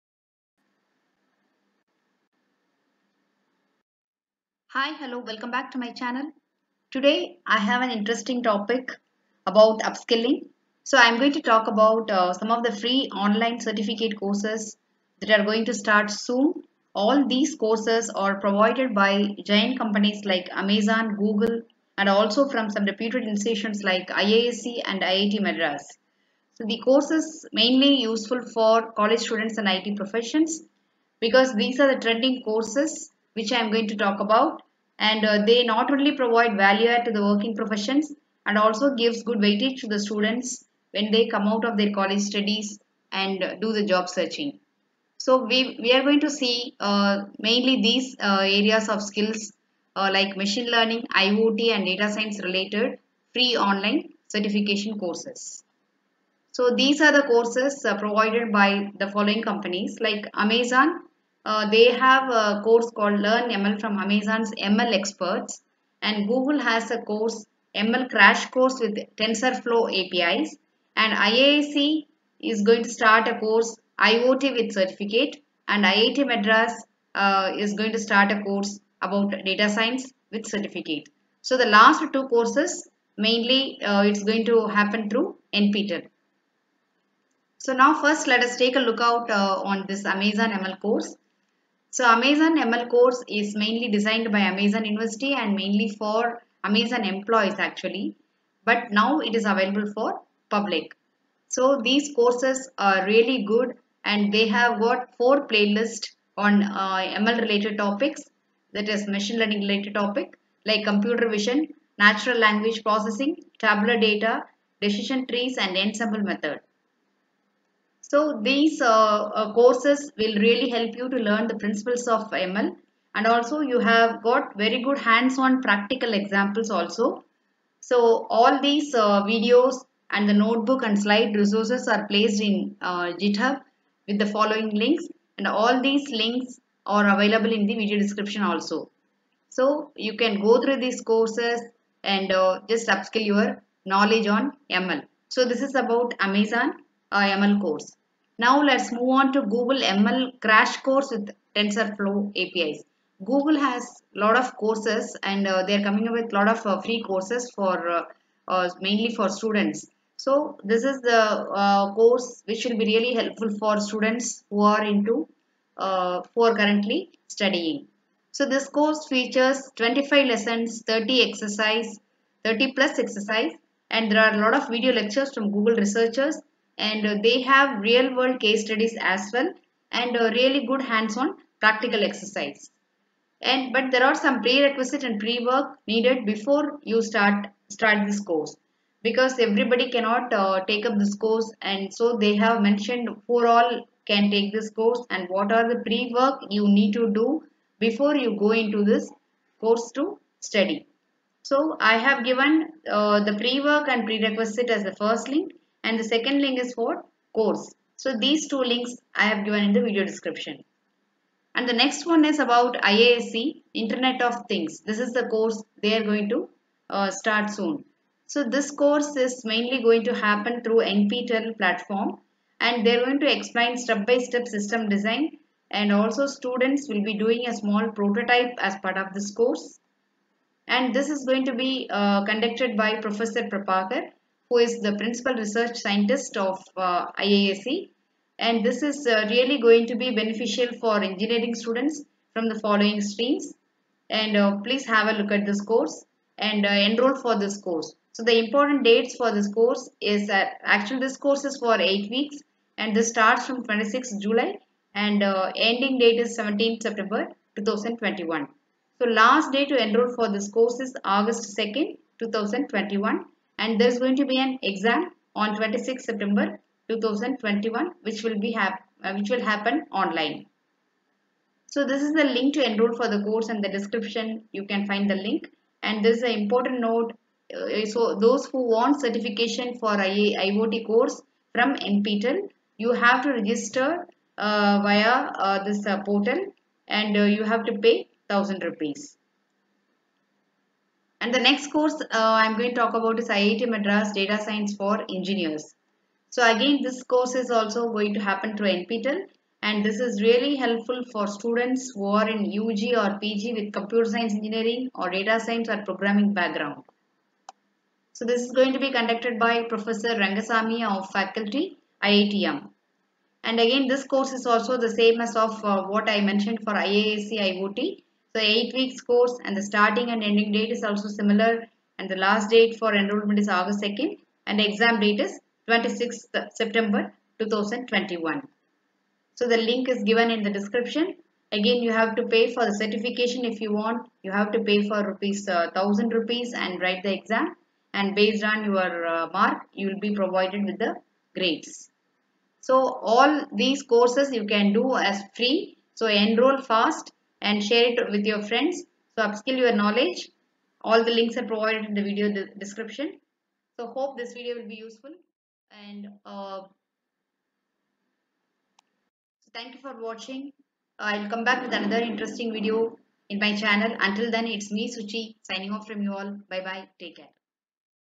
free certificate courses online 2021 | ML, Data Science, IOT | Amazon, Google, IISC, IITMadras
Welcome to SuchiTalks 2020!!
Timestamp:
1:28 : Amazon ML course Details
2:28 : Google ML Crash course
3:32 : IISC - Inernet of Things(IOT)
4:53 : IITM - DataScience for Engineers

Free Online Courses With certificates 2021

This video has some of the online certification courses
on the Trending technologies
1.ML
2.Data Science
3.IOT

**Courses are From AMAZON, Google, IISC, IITM

2.Amazon ML course

3.
Free (IOT) Internet of Things Course from IISc Bangalore with Certificate

4.IIT Madras has invited applications from interested participants for a free online course on Data Science for Engineers on the NPTEL platform